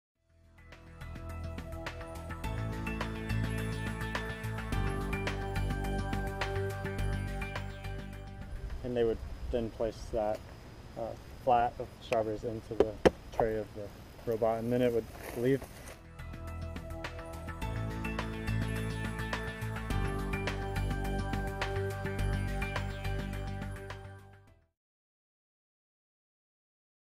Robotic Strawberry Harvest Aid
UC Davis graduate students Vic Jang and Garren Lewis brought their robotic strawberry harvesting aid to a nearby commercial strawberry farm to do some field testing on Friday, April 3, 2015. Their robotic research may help improve the efficiency and labor costs of strawberry harvesting.